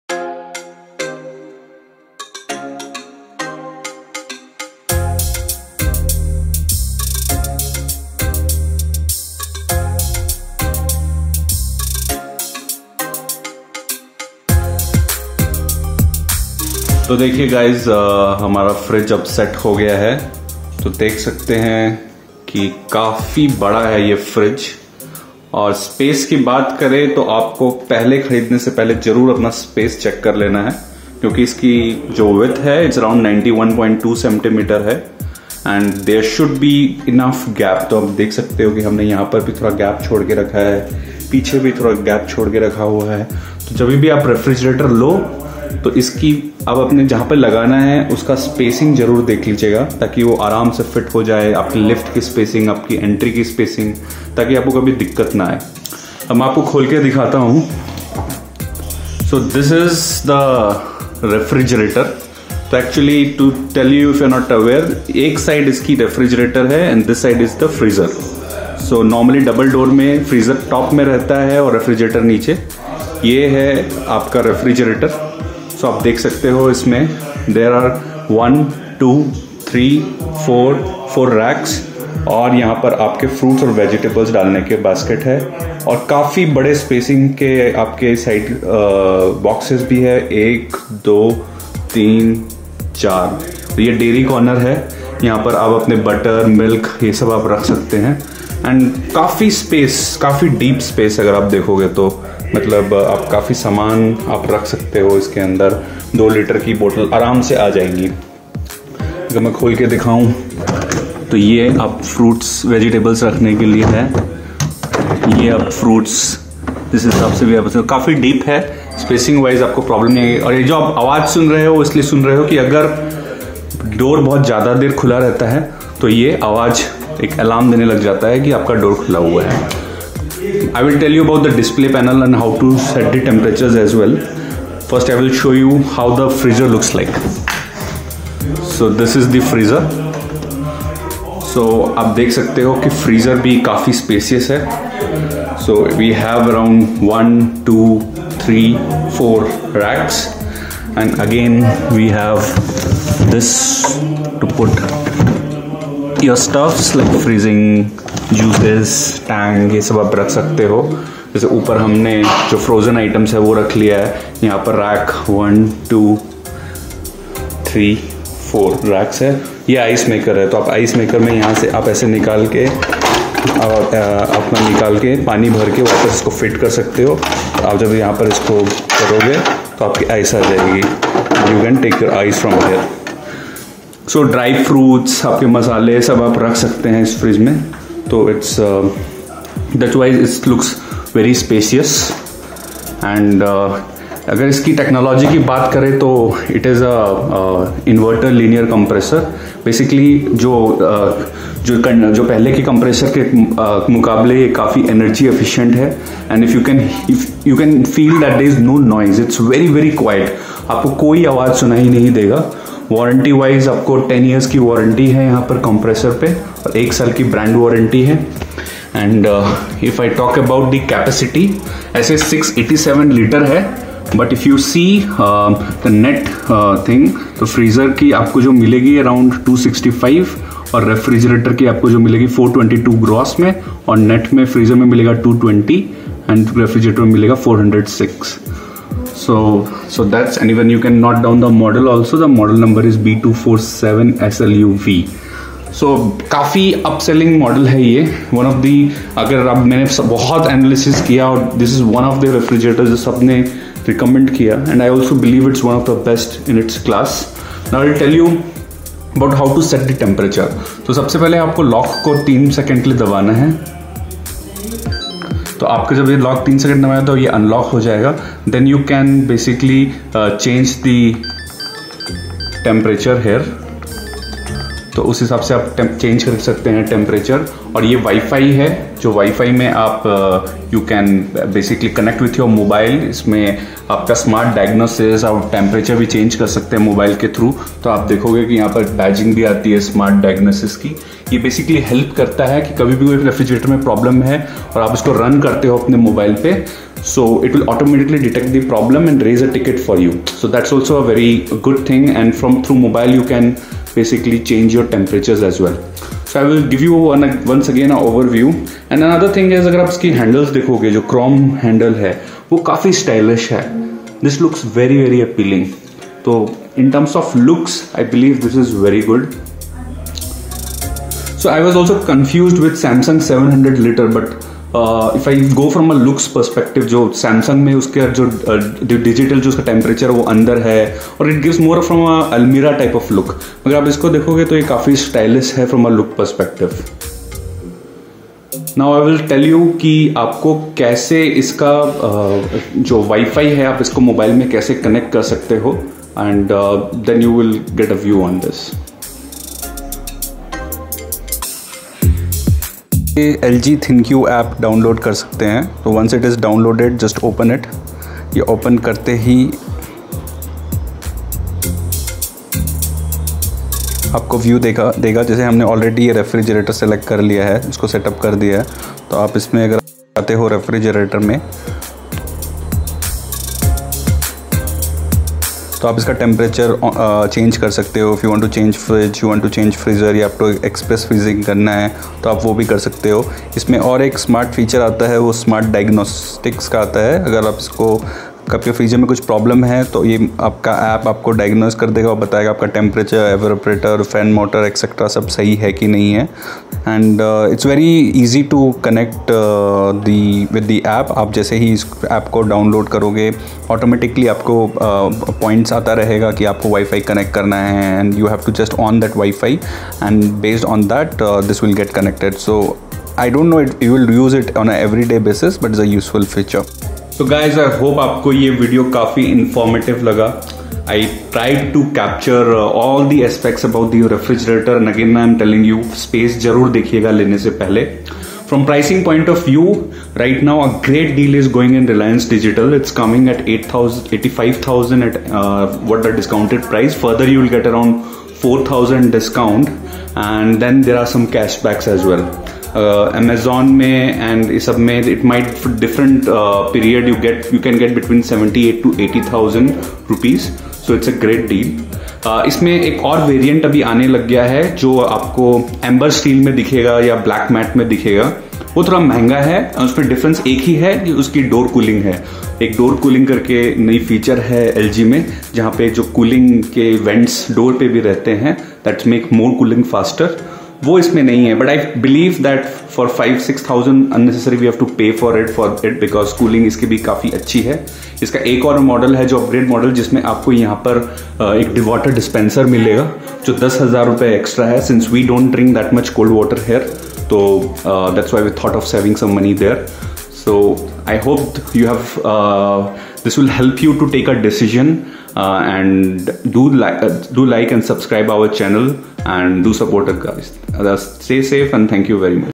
Review of LG 687 ltr frost free side by side Refrigerator B247SLUV
Review and unboxing of LG687 side by side refrigerator, price and discount details included in the video